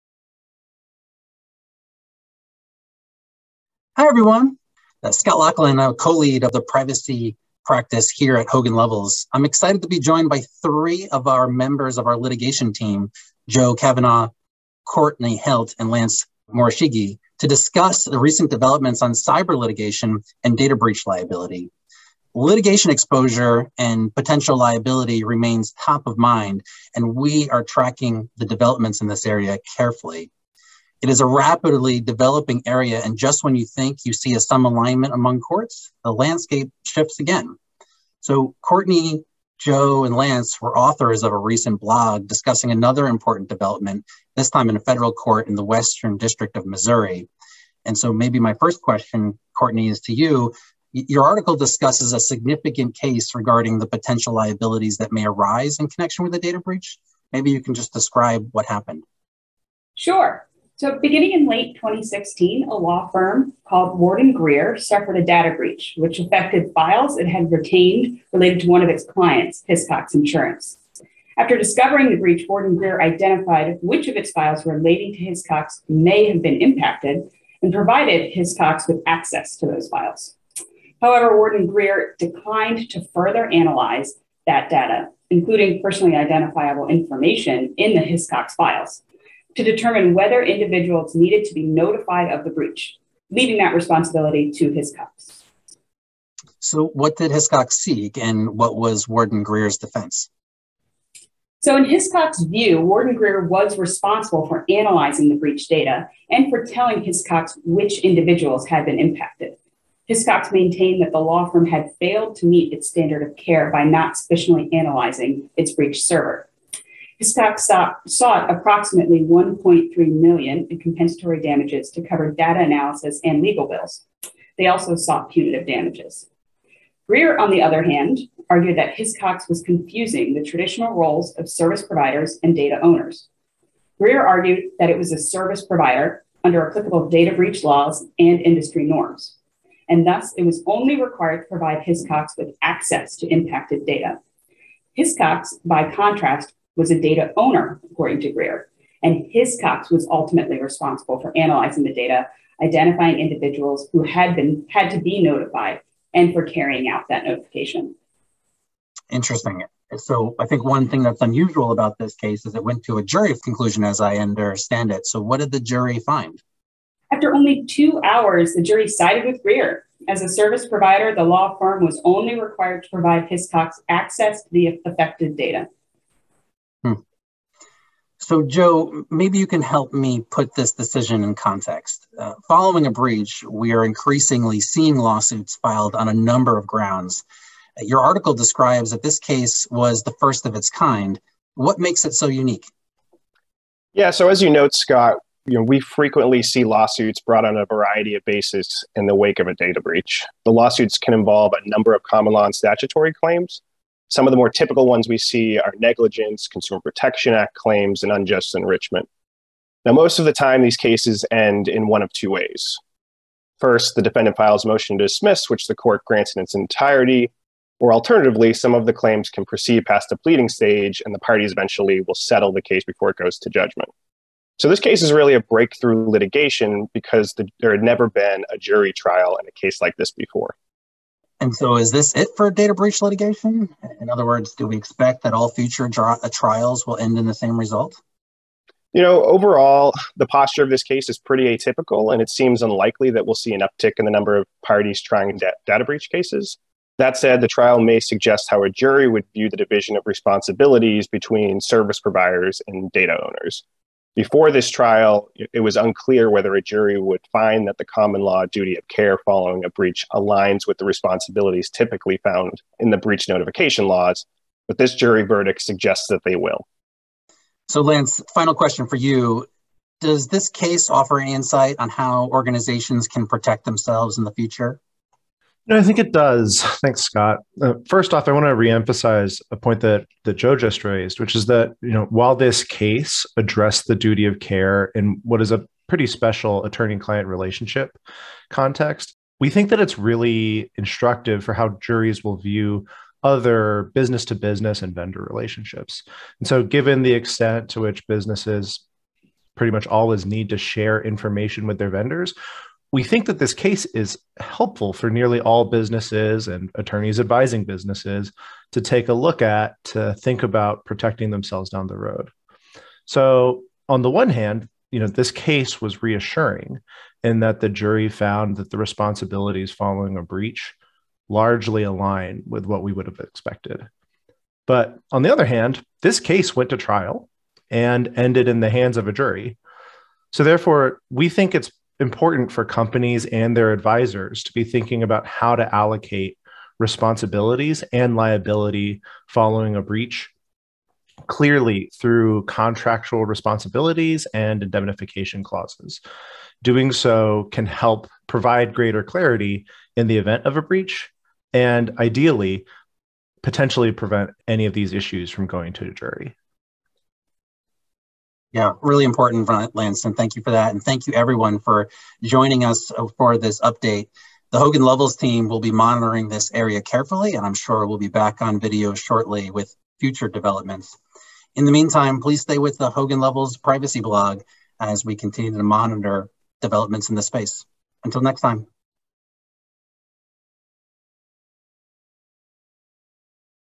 Data breach response liability: Jury finds defendant not negligent for response to data breach
This is a Chronicle of Data Protection Brief summarizing a Hogan Lovells publication.